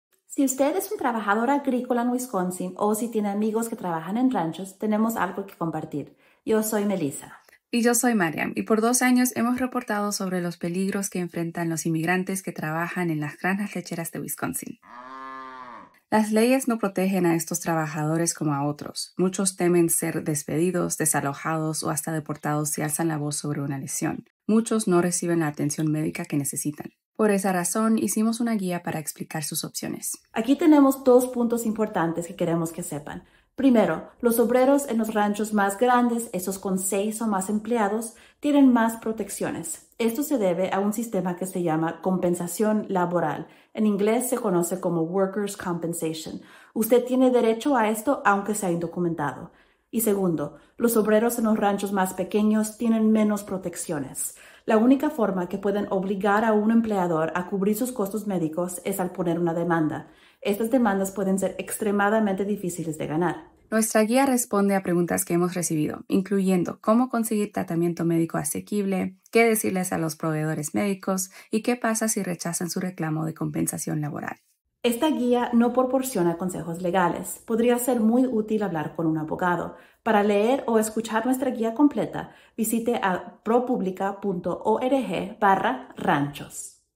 Qué hacer si se lesiona en el trabajo en una granja de Wisconsin
Durante los últimos dos años, hemos reportado sobre las vidas de los obreros de la industria lechera de Wisconsin y los peligros que enfrentan en el trabajo. Estos trabajadores están excluidos de muchas de las protecciones legales del estado y del gobierno federal que ayudan a otros trabajadores. Pocos de los obreros que entrevistamos entendían sus derechos después de una lesión. Por esa razón, hicimos esta guía para explicar sus opciones. Aquí te dejamos saber lo más importante si trabajas en un rancho o conoces a un trabajador agrícola.

📰: Melissa Sanchez y Maryam Jameel
📸 y 🎨: Cuauhtémoc Wetzka, Zeke Peña, Edel Rodriguez, Sebastián Hidalgo
🎥: José Sepúlveda

ProPublica es una redacción independiente sin fines de lucro que produce periodismo de investigación con fuerza moral.